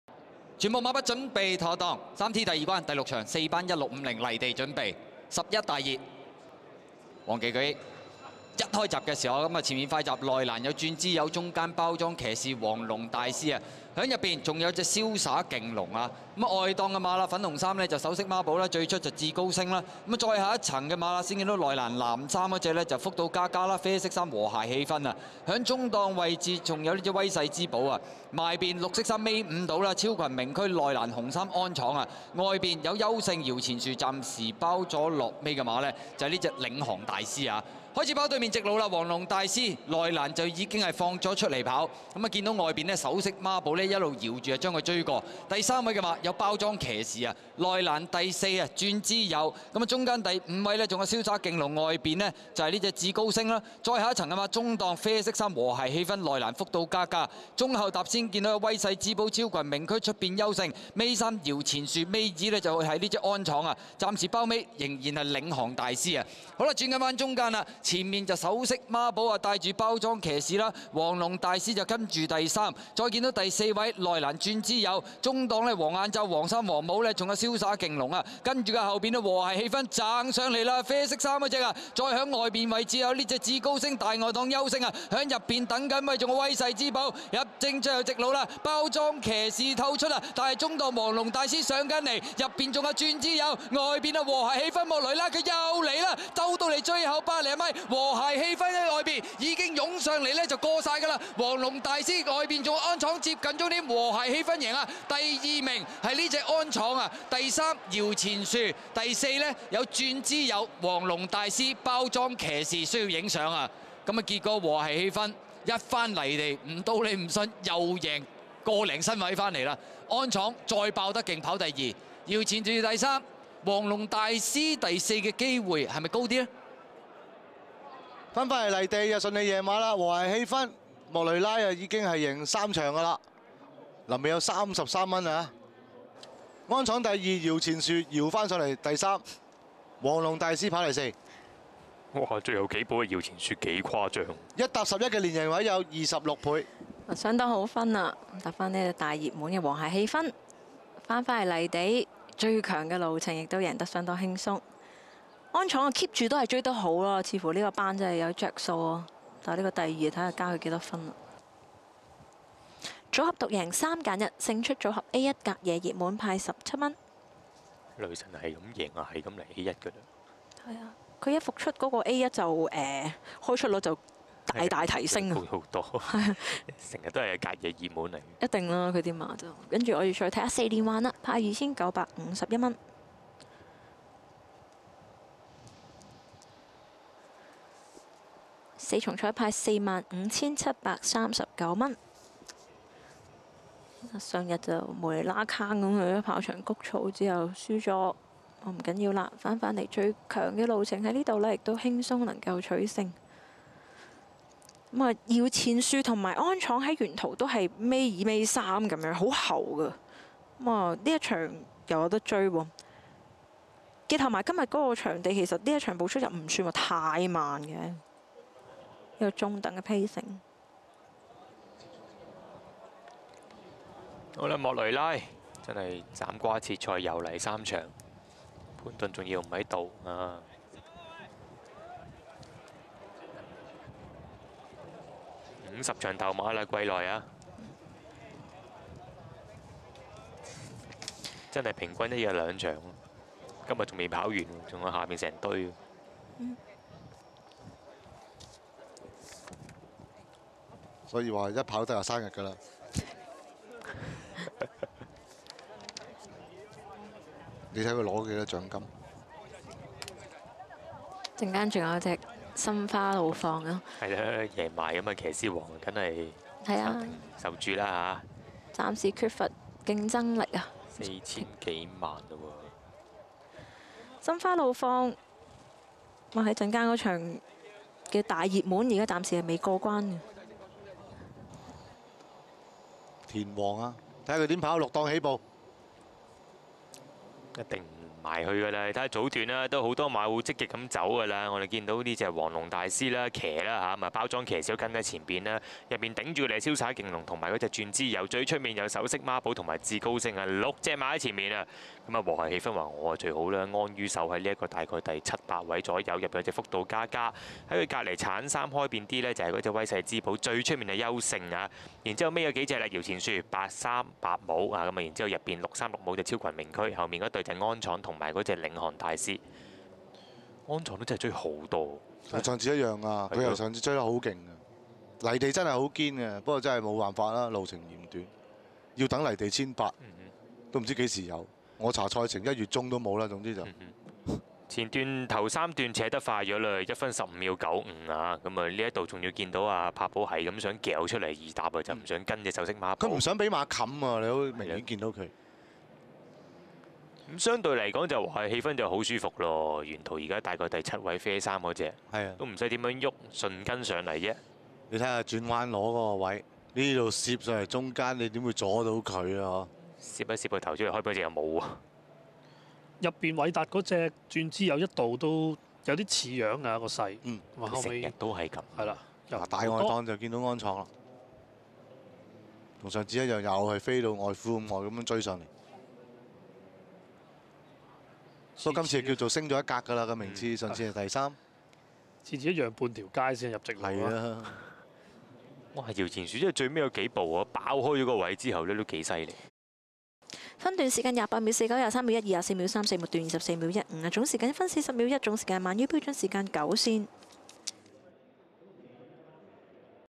和諧氣氛 (S326) 莫雷拉 06/12/2015
06/12/2015 沙田 (場次229)
第四班 - 1650米 - (60-40) - 漢口讓賽
場地狀況 :  好地
賽道 :  全天候跑道
騎師 :  莫雷拉